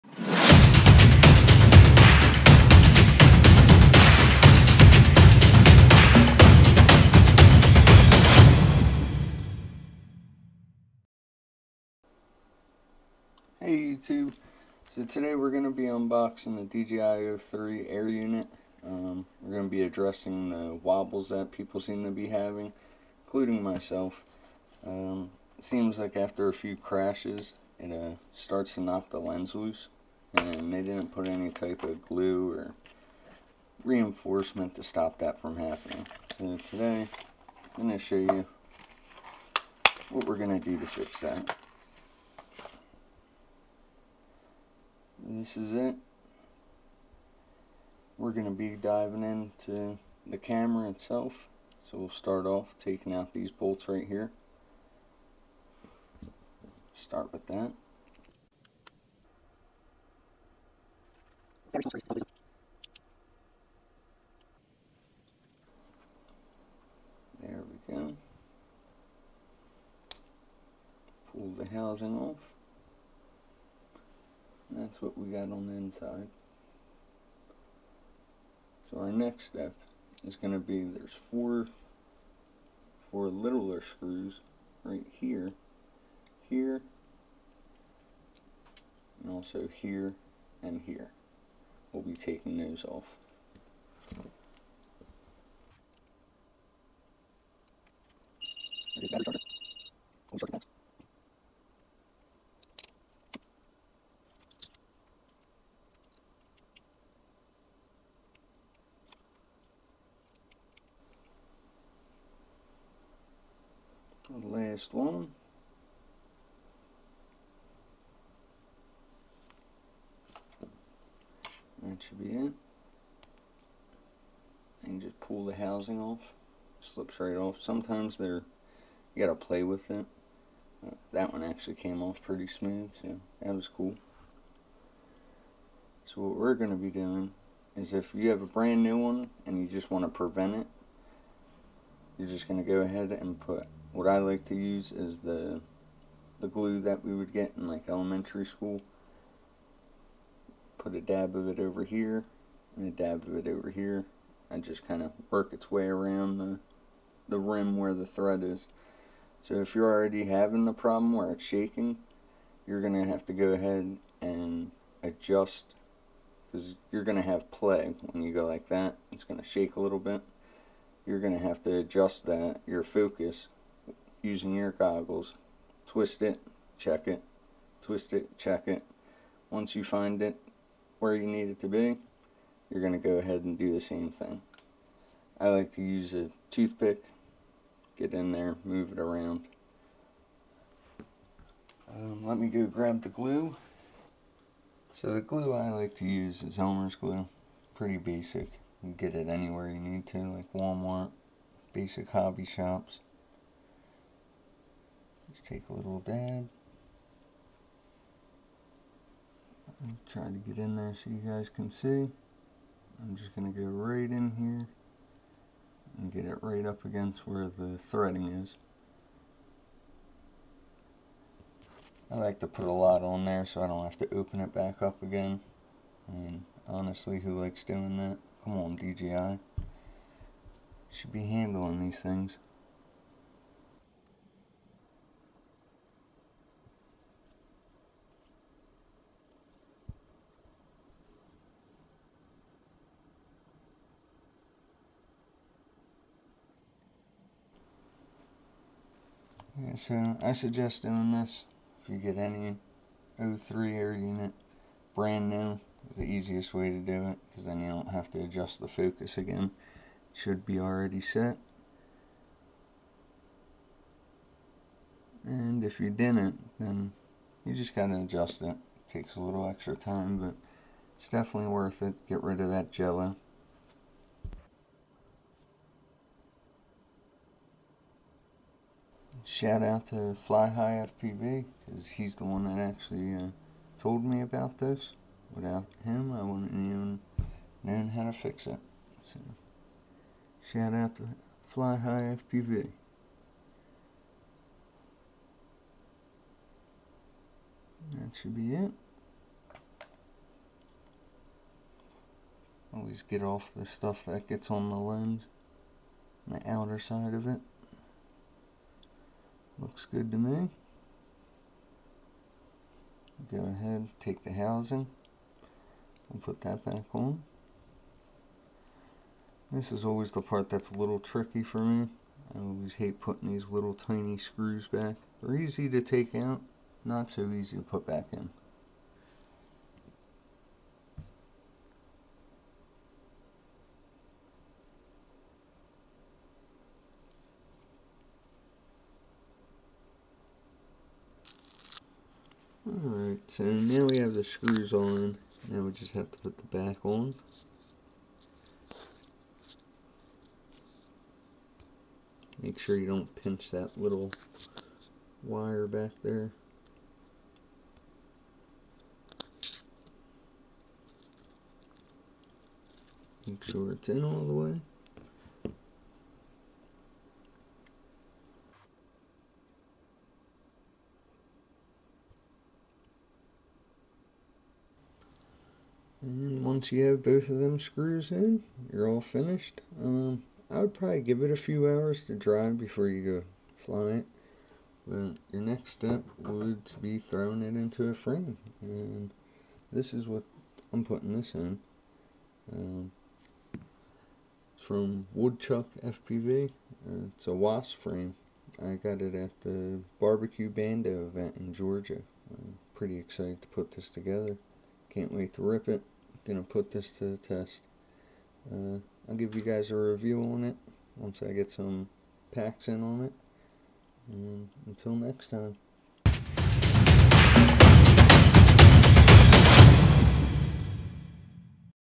How to Fix DJI o3 Air Unit Video Shake/Wobble
Fixing the issue with the dji o3 video wobbles